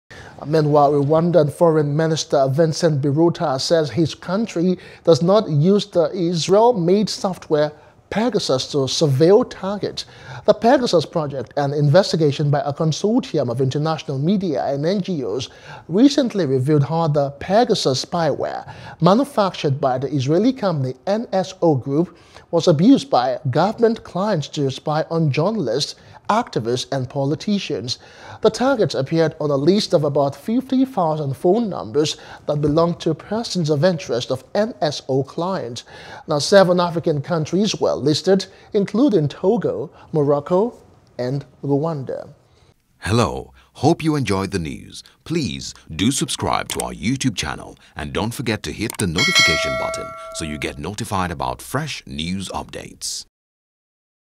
Rwanda Denies Using Pegasus Spyware | AFRICAN
Meanwhile, Rwandan Foreign Minister Vincent Biruta says his country does not use the Israel-made software Pegasus to surveille targets.